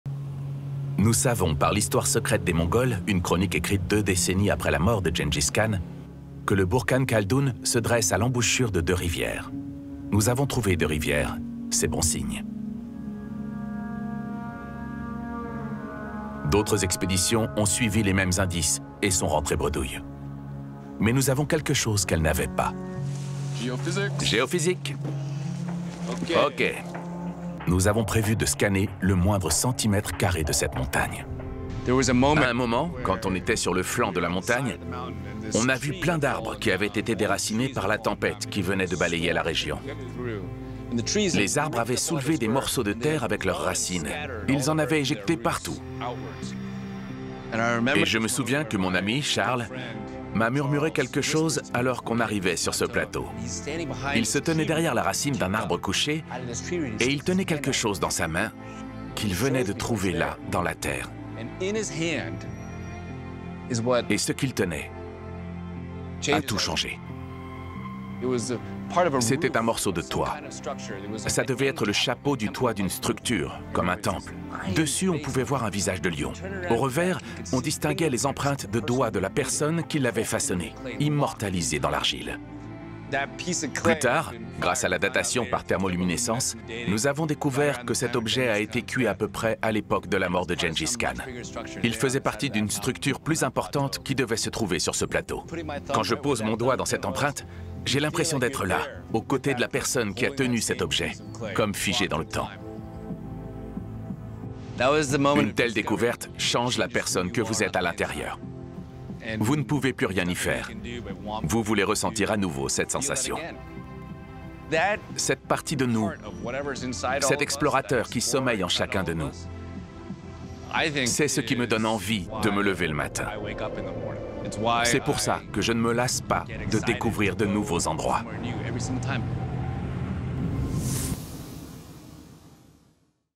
Découverte d'un objet datant de l'époque de Gengis Khan
Cette équipe d'explorateurs et d'archéologues fait des recherches au pied du Burkhan Khaldun, en Mongolie.